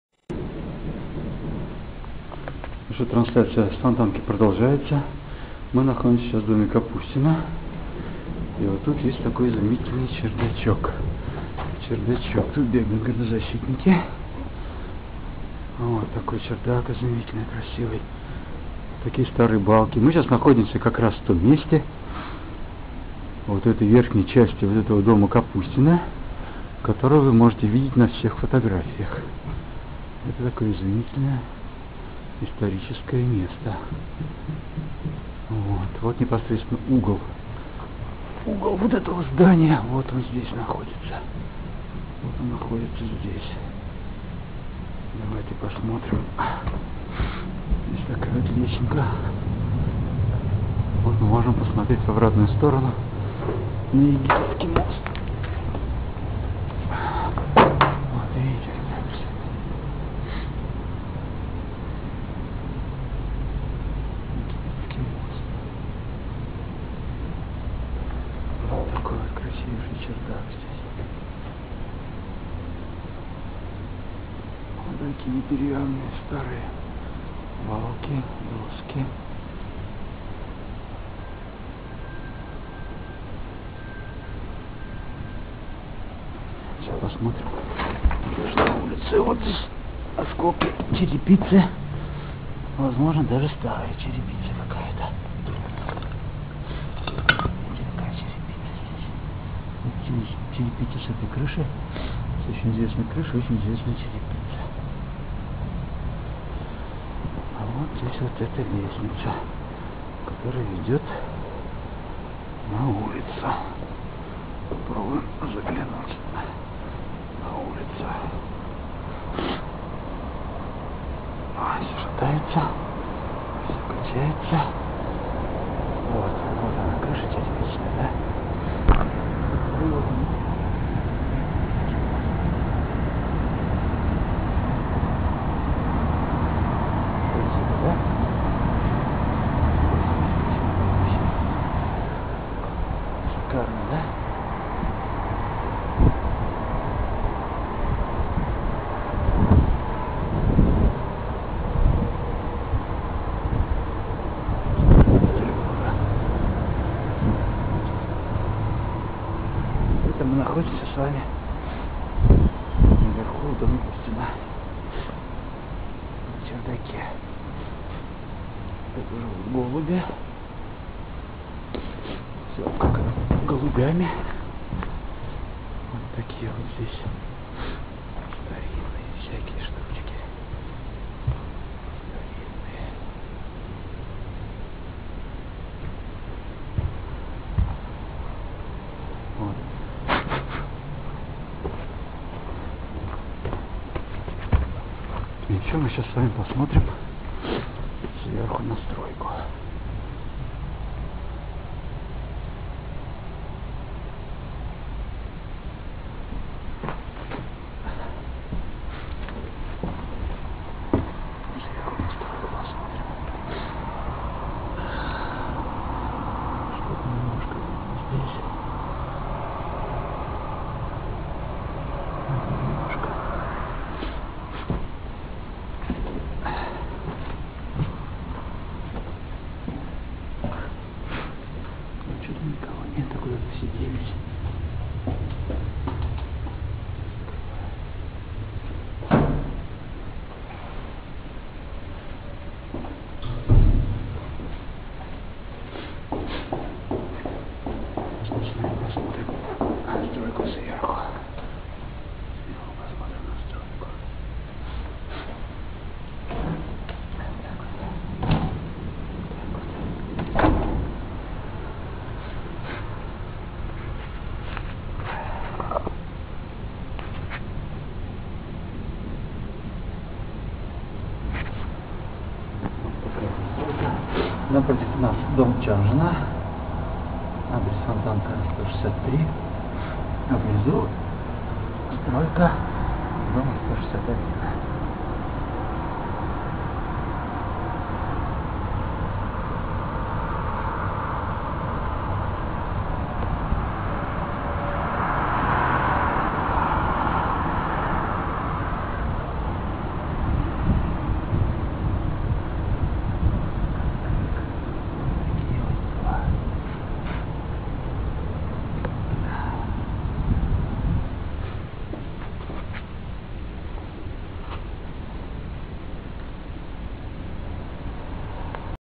10 !!! Пикет у Фонтанки 161. Петербург. 21.01.12.
На чердаке дома Капустина! Вид с крыши на Египетский мост! Вид на стройку сверху.

Жители домов 159, дом Капутина, и 163, дом Чанжина, по Фонтанке вышли в протестный пикет против строительства дома по адресу Фонтанка 161 с нарушениями. Застройщик хочет нарушить регламент и построить дом в притирку к существующим домам. При этом образуются дворы-колодцы и возникает угроза парятникам, стоящим на охране.
В пикете приняли участие активисты Живого ророда.

Picket defenders of the city near the Fontanka 161. St. Petersburg.